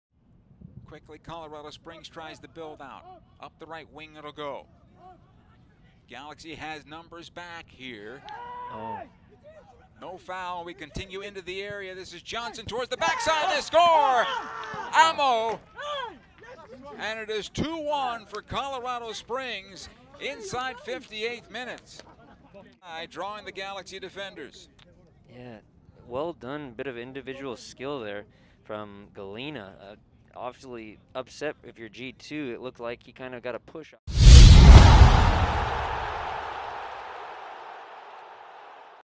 Elvis Amoh with a Goal vs. LA Galaxy II
Elvis Amoh (Colorado Springs Switchbacks FC) with a Goal vs. LA Galaxy II, 06/11/2022